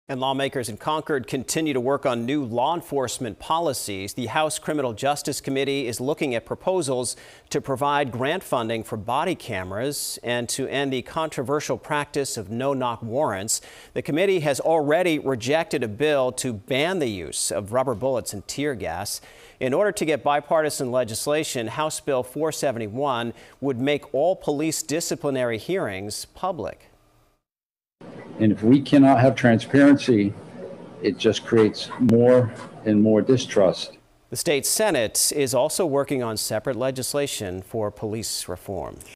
Hearings held on law enforcement reform legislation
Lawmakers in Concord are considering bills to encourage the use of police body cameras and end no-knock warrants, among other law enforcement reforms.